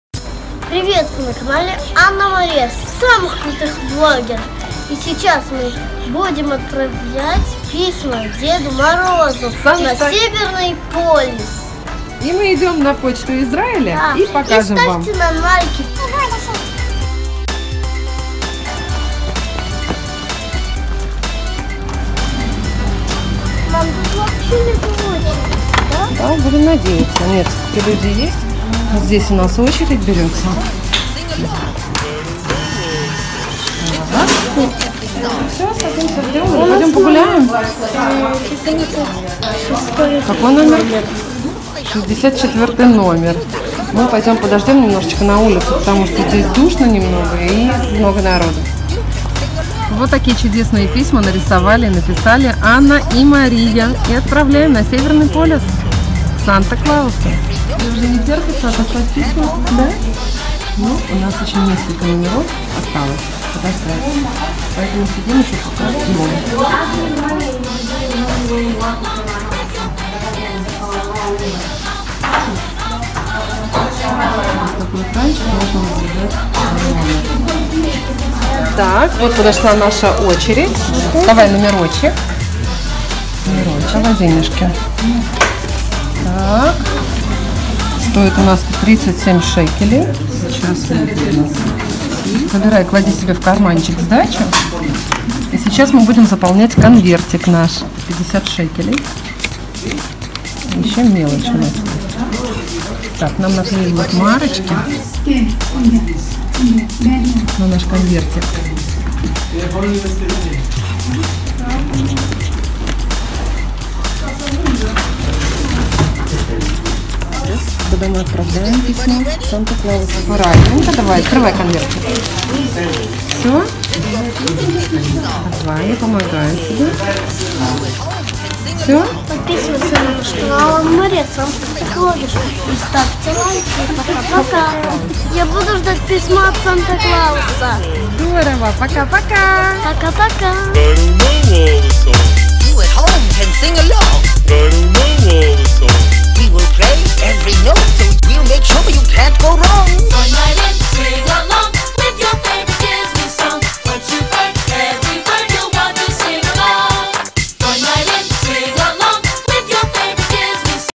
North Pole ❄️ To Santa Claus Letter почта Израиля Письмо Санта Клаусу ANNA MARIA
Send a letter to Santa Claus NORTH POLE! Dreams*gifts
Post ISRAEL

Отправляем письмо Санта Клаусу в Канаду!
Мечты*подарки
Почта Израиля

❤️ ANNA MARIA ❤️